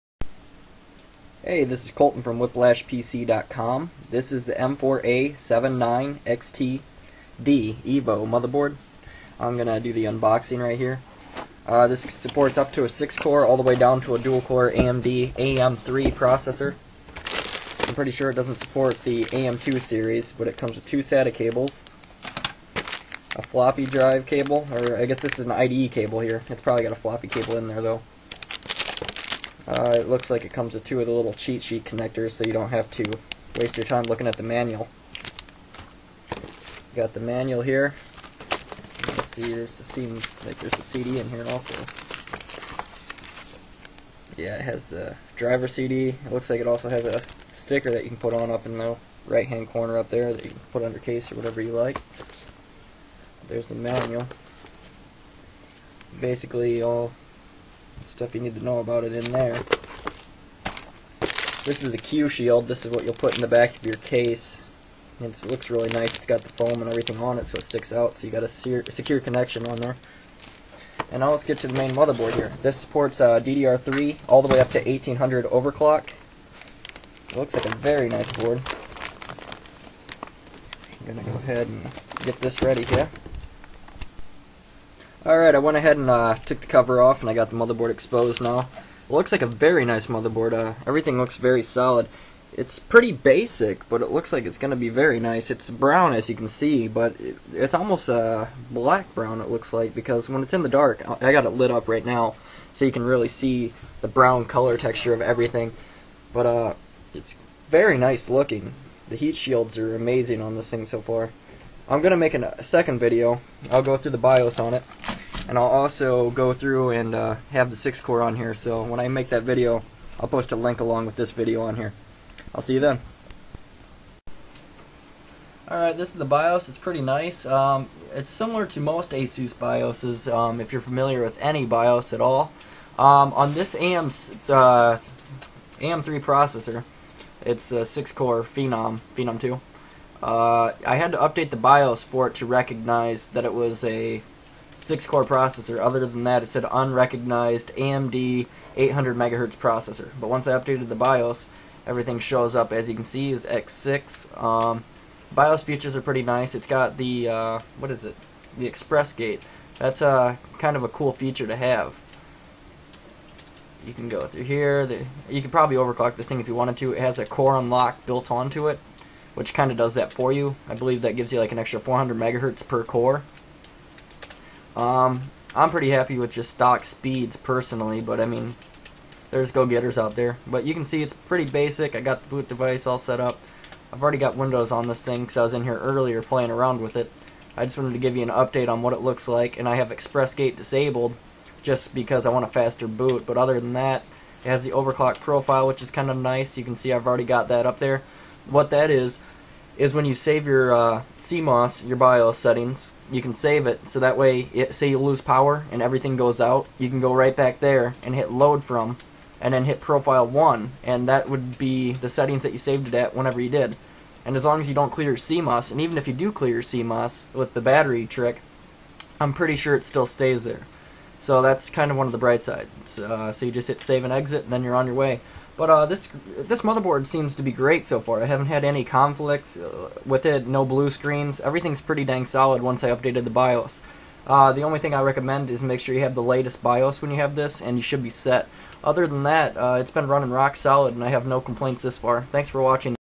Unboxing : ASUS M4A79XTD EVO AM3
Today we're unboxing the ASUS M4A79XTD EVO AM3
This motherboard is the best bang for the buck in the market in my personal Opinion.
This motherboard is rock solid with great features and a beautiful design.

Included is also a walk through the bios and some of it's features.

Whiplashpc.com for all your computer needs.

Enjoy in 720P Quality!